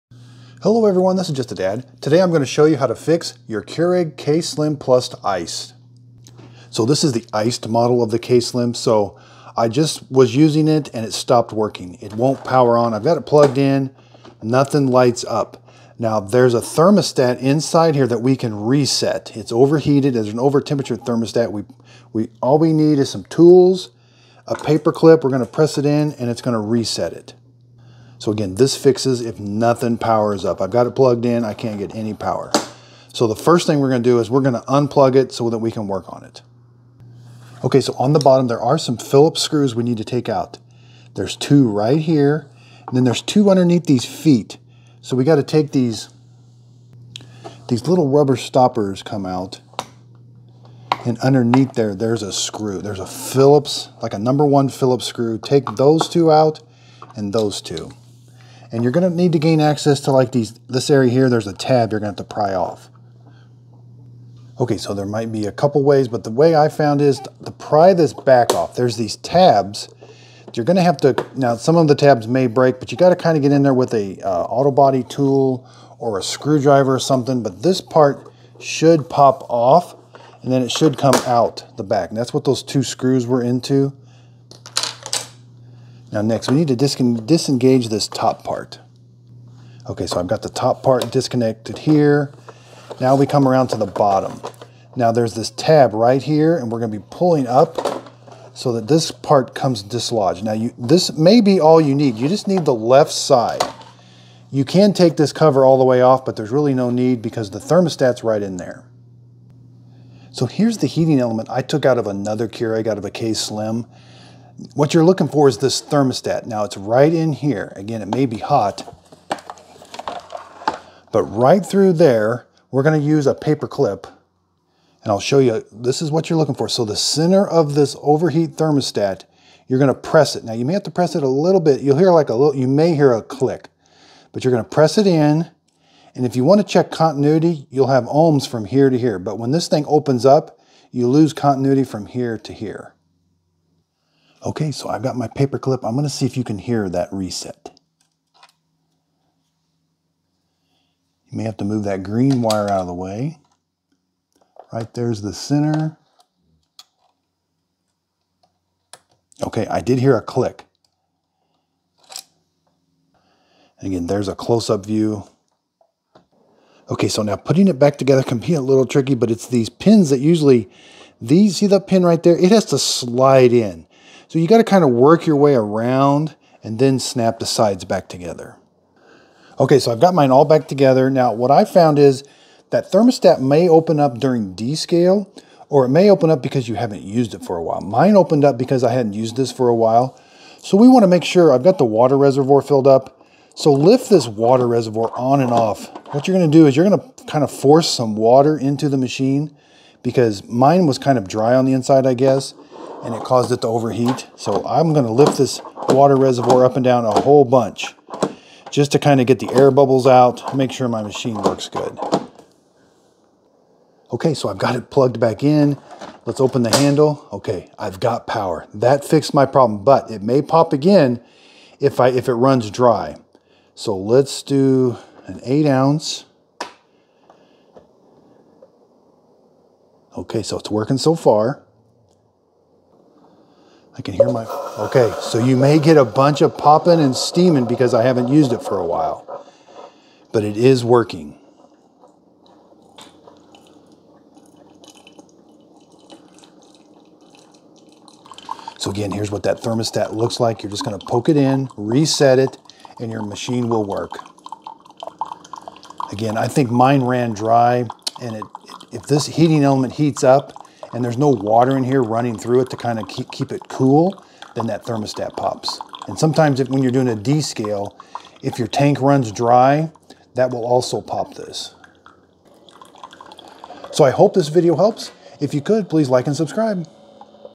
How To Fix Keurig K-Slim + Iced Coffee Maker No Power RESET THERMOSTAT
How To FIX / Reset the thermostat on Keurig K-Slim Plus Iced Coffee Maker model K191.  How to fix dead no power by resetting it.   How to open it up and use a paper clip to reset it.  what tools you will need and how to prime the pump to prevent it from happening again.